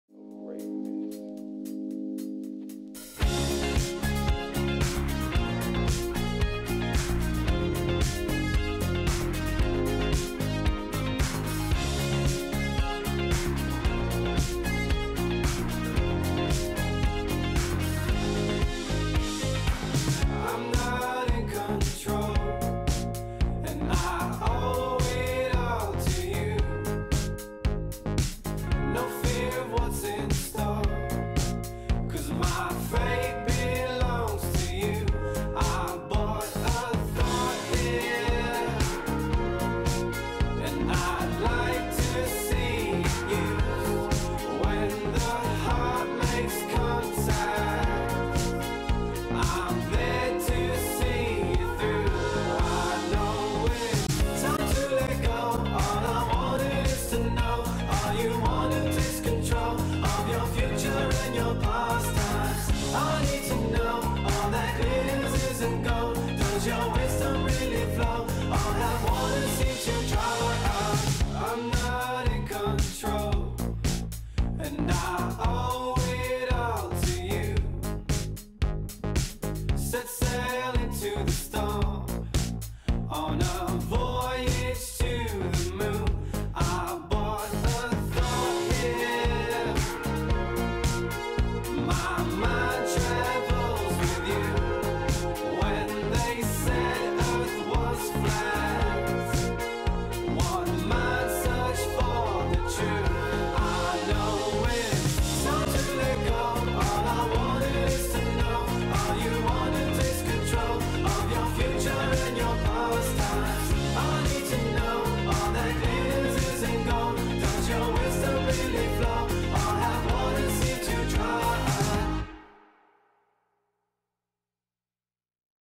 Motion GFX Billboard Reel
I want to break into the advertising industry so I thought I’d highjack everyone’s billboard space! :)